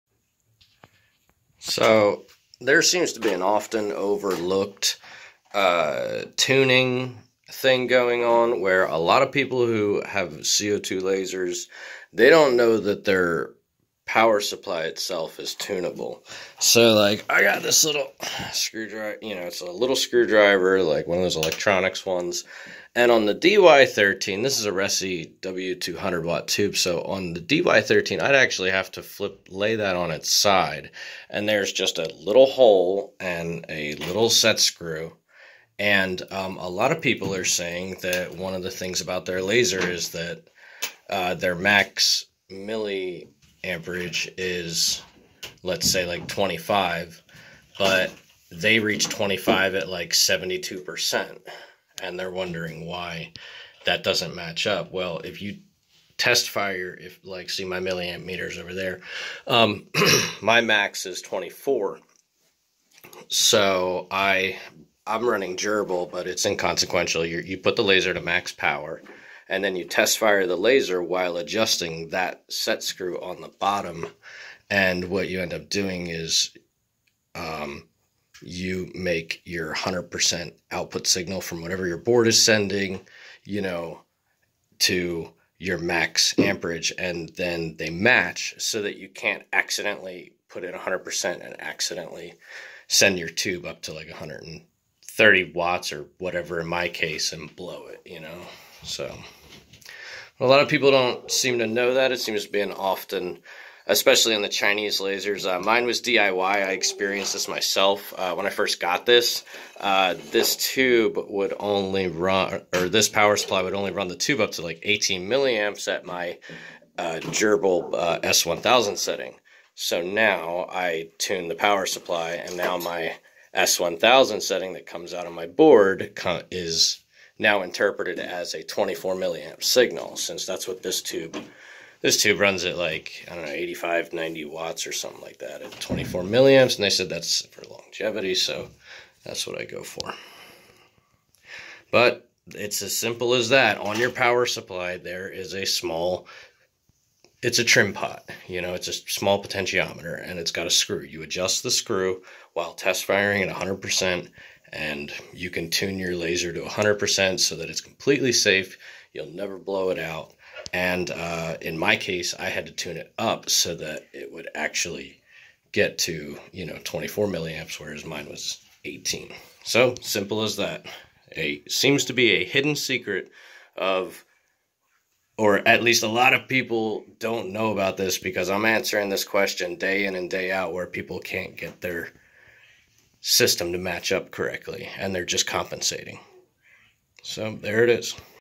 Tuning your co2 laser psu to your tube so you dont fry your tube. Reci w2 100 watt dy-13 psu dy13
Ive often seen people who can only run their laser at say 71% power or they will put too much current to the tube, risking damage. This will explain how to tune your laser psu (most if not all laser psus have this adjustment) to the tube, so that 100% power is the max current you desire to send to the tube. Dont accidentally fry your tube simply because your psu isnt tuned to the tube.